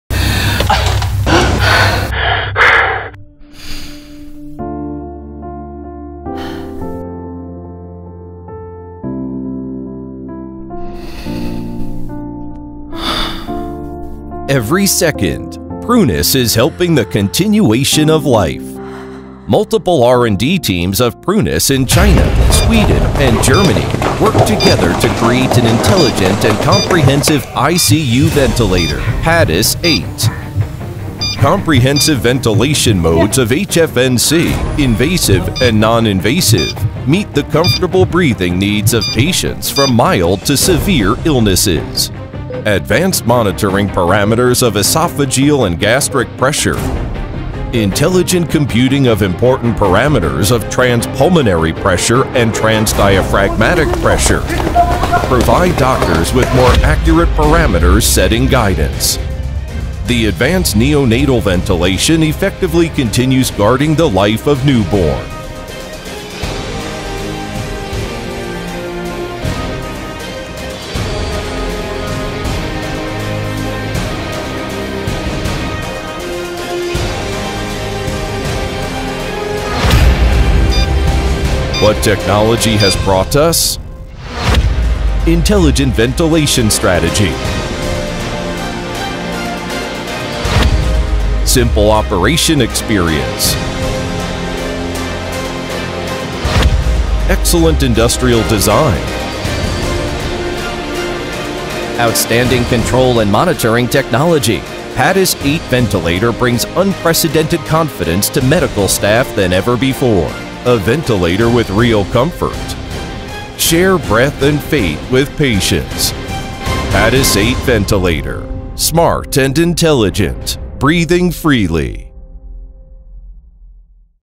PRUNUS Padus Ventilator Intro
Top-class certified quality | Outstanding performance | Superior safety

PADUS 8 VENTILATOR

+ The next generation of intelligent ICU ventilators
+ Innovative technology developed and sourced from top European professionals and Shenzhen R&D team.
+ Padus 8 ventilator : one of the most advanced intelligent ICU ventilator around the world.

        Intelligent experience that exceeds your imagination

    ... just what you have been looking for